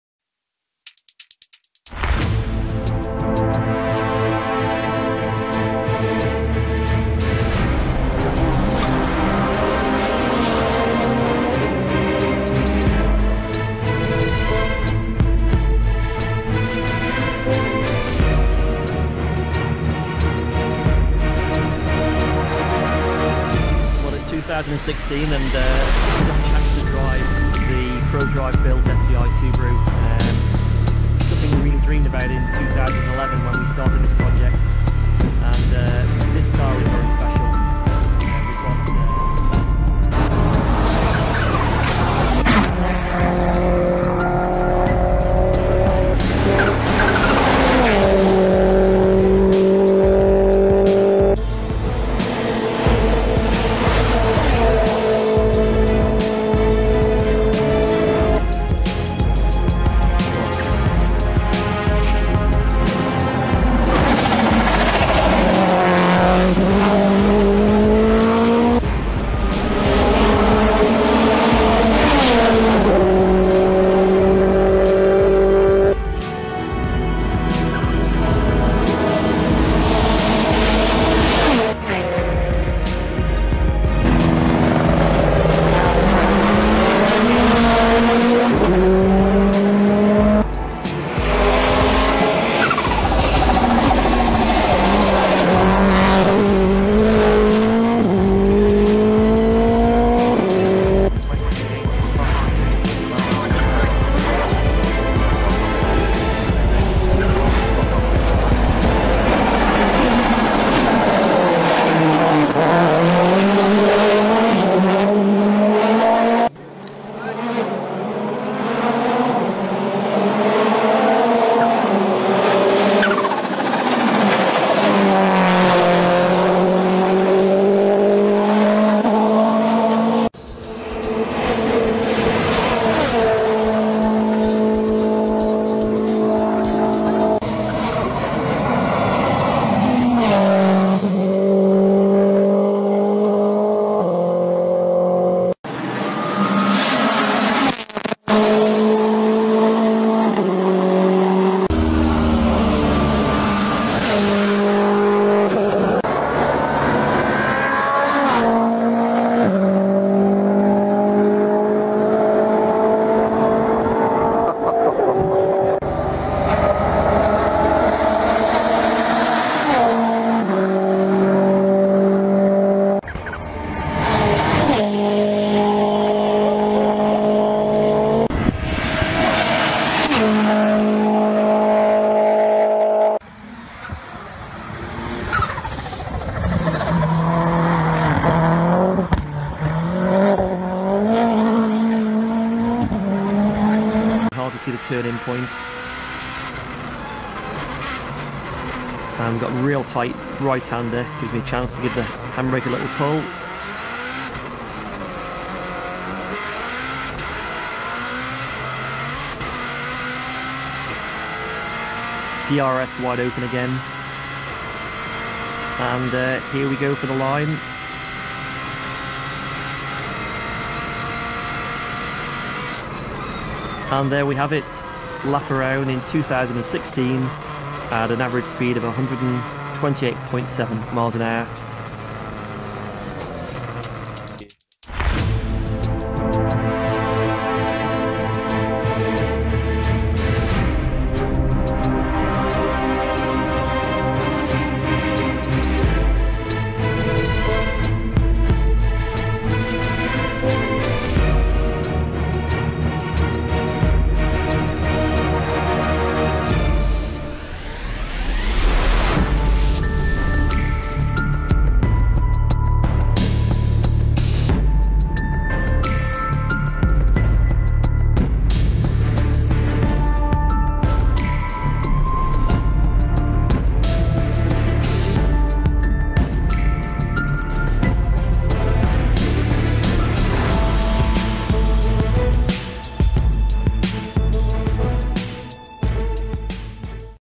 スバルWRX STIがマン島を全開走行狂気の走りが凄すぎる720p
ASCIONE Automobile
SUBARU Corse
Lana ASCIONE
Lana dekeersmacker